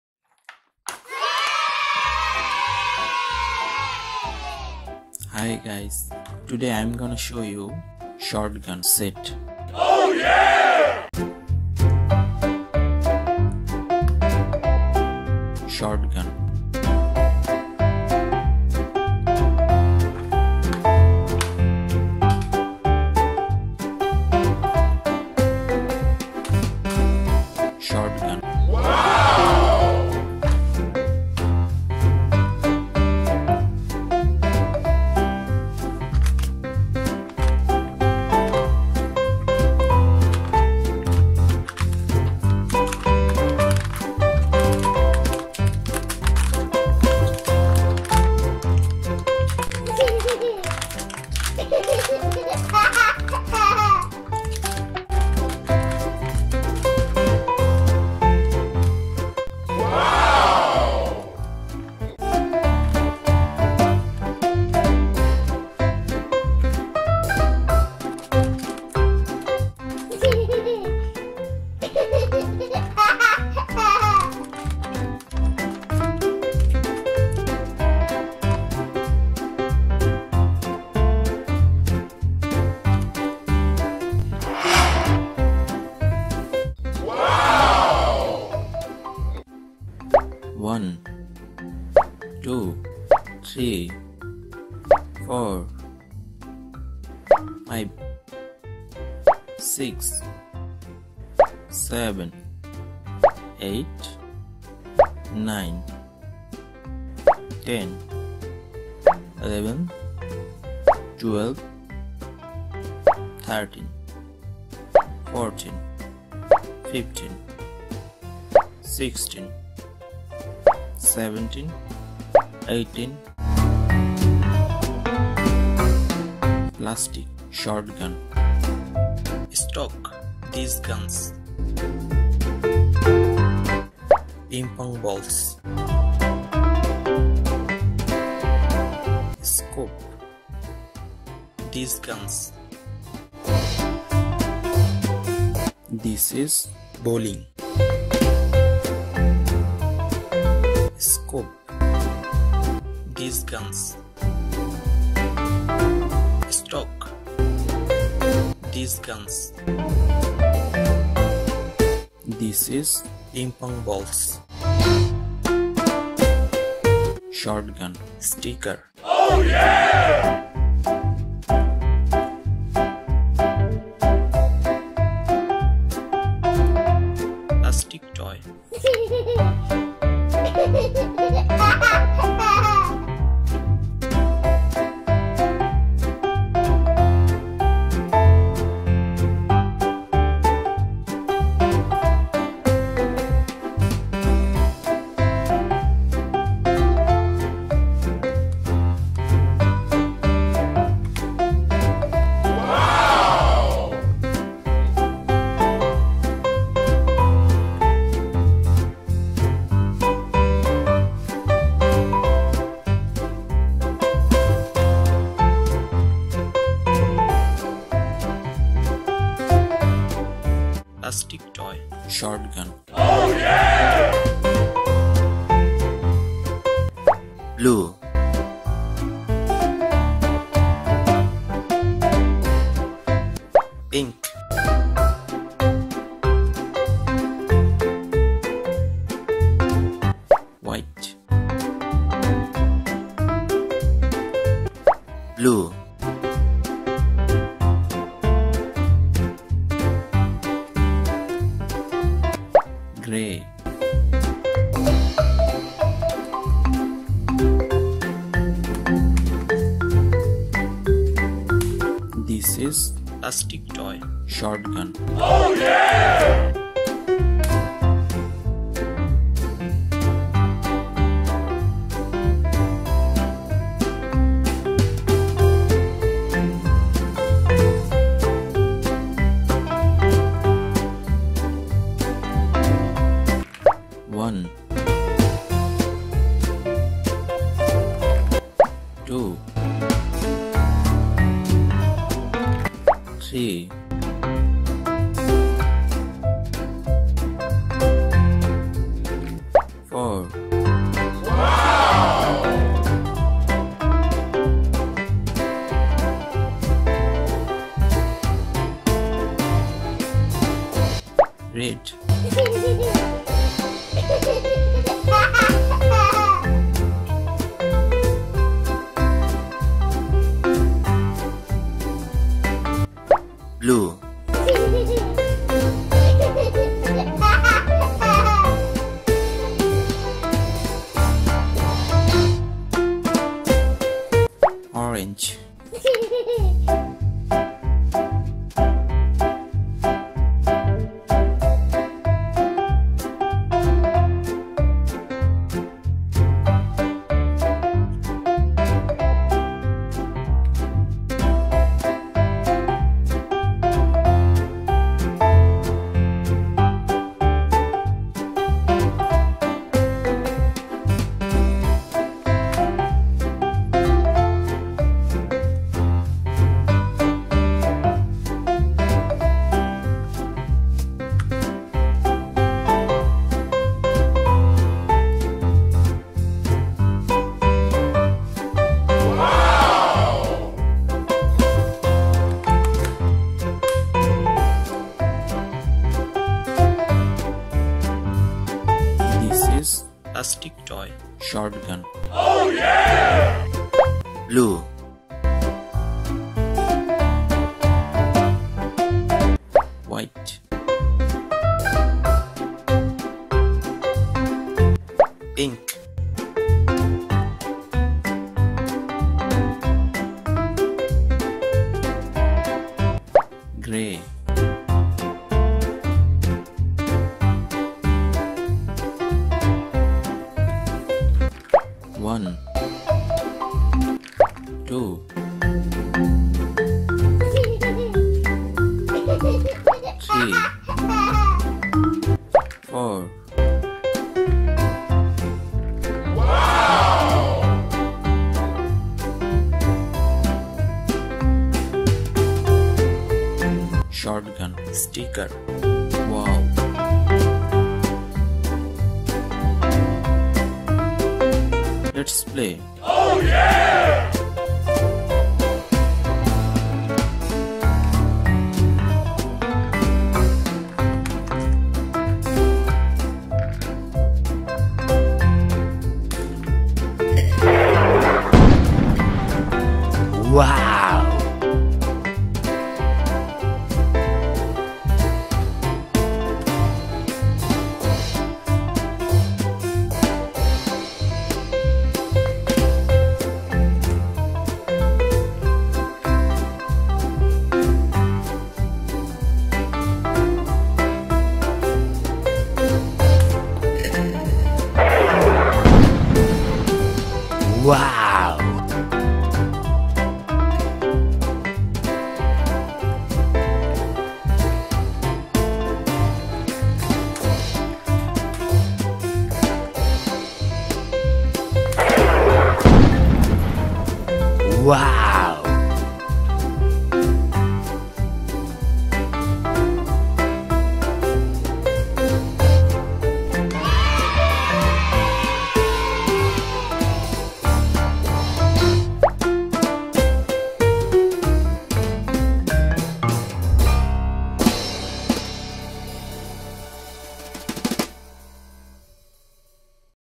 Shoot Toy Guns With Scope, Ping Pong Color Balls, Bowling and Shot Gun Sticker
★★★ Shotguns Set ★★★
1. Color Shoot Guns
2. Stoke
3. Scope
4. Ping Pong Ball
5. Bowling
6. Shot Gun Sticker